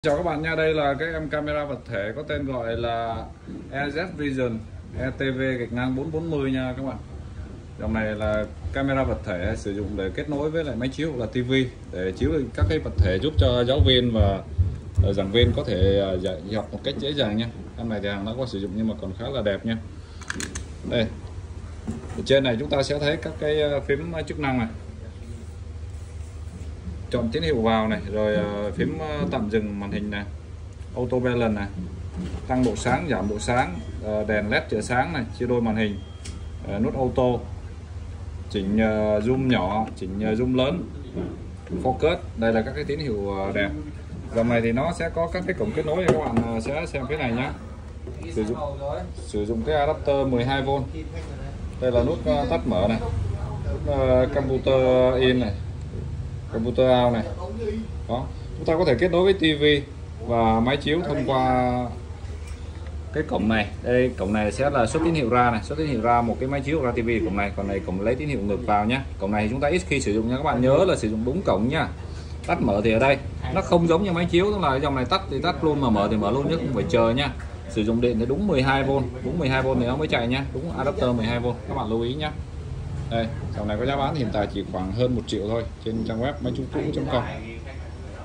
Camera vật thể cũ hàng qua sử dụng giá chỉ hơn 1tr EzVision ETV-440
Camera vật thể là gì và khi nào nên mua để đạo tạo và dạy học.

Việc thể hiện các hình ảnh kỹ thuật số bằng các nguồn có sẵn file trên máy tính, laptop hay điện thoại để phục vụ mục đích dạy học, nghiên cứu và thuyết trình bài học, dự án đã trở nên khá phổ biến hiện nay.

Tuy nhiên, để mô phỏng 1 hình ảnh thực tế lên 1 màn chiếu hoặc tivi lớn để nhiều người có thể dễ dàng quan sát được thì chúng ta lại cần đến 1 thiết bị thứ 2: Camera vật thể.

Vì lý do đó, sự phổ biến của Camera vật thể trở nên thịnh hành và phổ biến.
So với giá thành của một máy chiếu, tivi thì camera vật thể có giá rẻ hơn nhiều, trung bình chỉ từ 4-10 triệu là bạn có thể sở hữu 1 camera vật thể mới.

Một số hãng phổ biến như Epson, AverVision, Optoma, Toshiba...
Cách sử dụng rất đơn giản:

1. Kết nối Camera vật thể với máy chiếu hoặc tivi thông qua cáp HDMI hoặc VGA
2. Bật camera vật thể và để hình ảnh, tài liệu và vật dụng trước ống kính camera và chỉnh zoom / focus cho phù hợp.

Hình ảnh sẽ được truyền và phóng lớn trên màn hình máy chiếu, tivi giúp người xem quan sát rõ hơn, giúp bài dạy học, thuyết trình hiệu quả hơn.

Một số camera vật thể giá chỉ hơn 1trx bạn có thể tham khảo thêm tại

Etv440
Ezvission etv-440